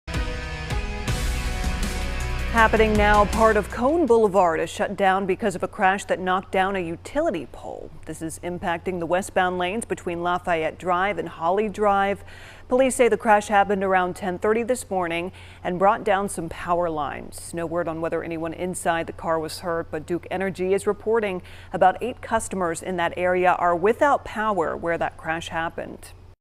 Crash involving utility pole closes down part of Cone Boulevard in Greensboro
The city of Greensboro says a crash has closed the westbound lanes of Cone Boulevard between Lafayette Drive and Holly Drive. The crash involves a utility pole.